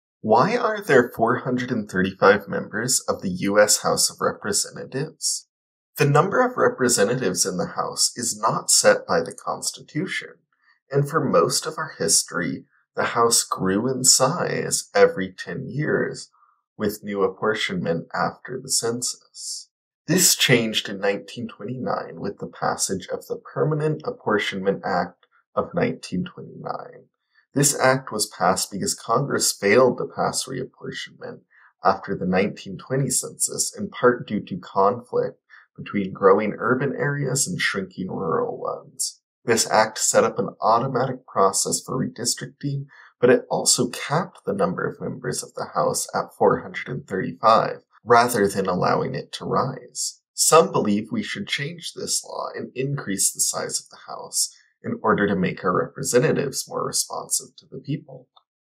Why are there 435 members of the US House?
The Us Constitution does not specify how many members of the House that there should be so were did the number 435 come from. This video takes a look at that question.